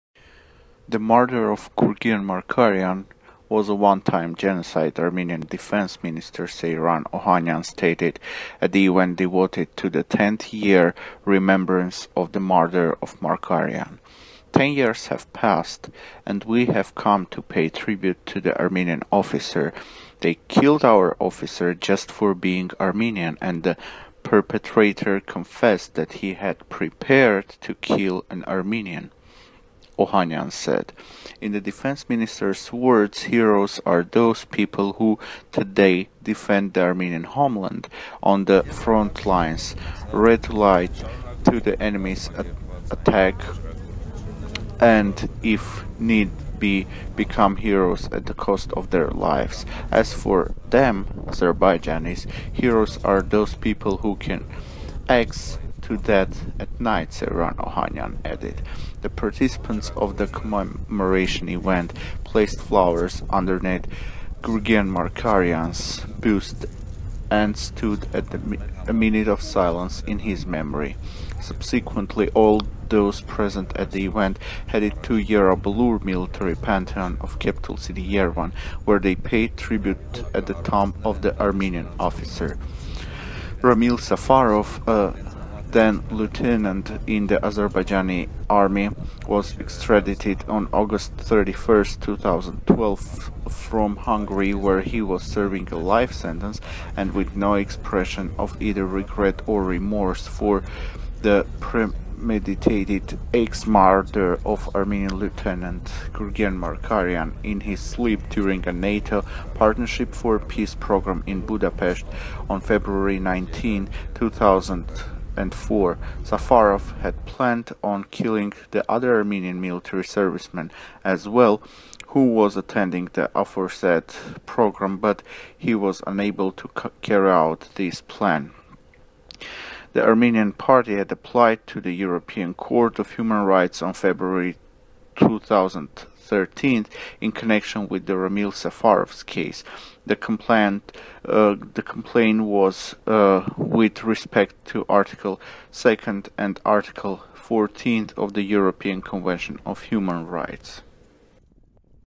Armenians remember Gurgen Margaryan - victim of Azeri killer - Feb 19, 2014
The murder of Gurgen Margaryan was a one-time genocide.